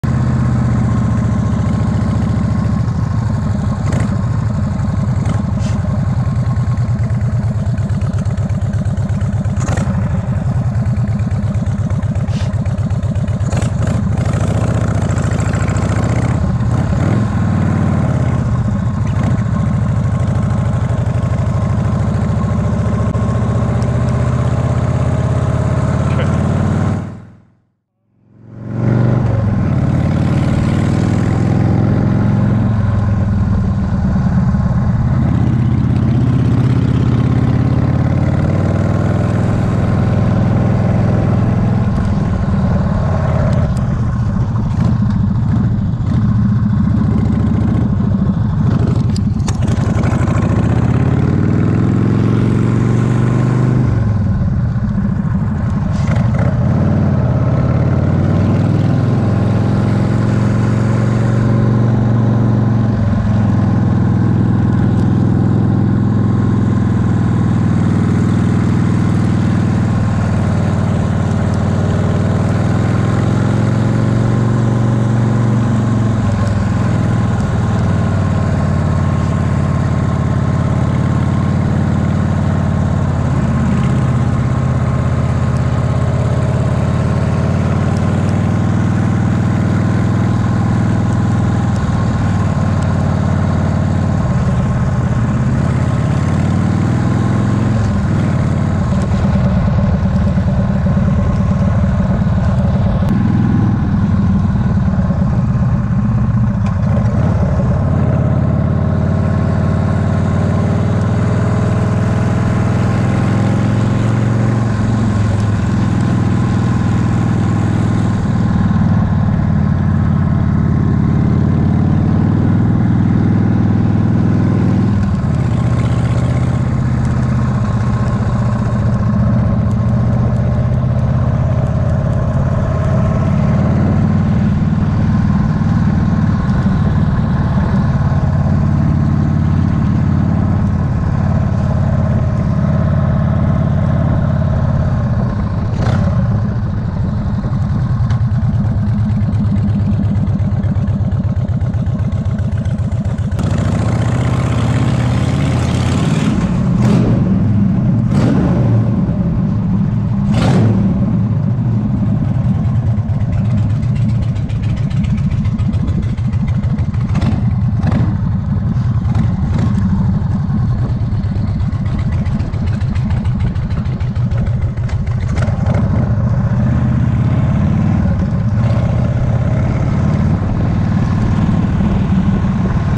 Riding Needles Highway - Sturgis Rally 2019...
Riding Needles Hwy in Custer State Park - Sturgis Rally 2019.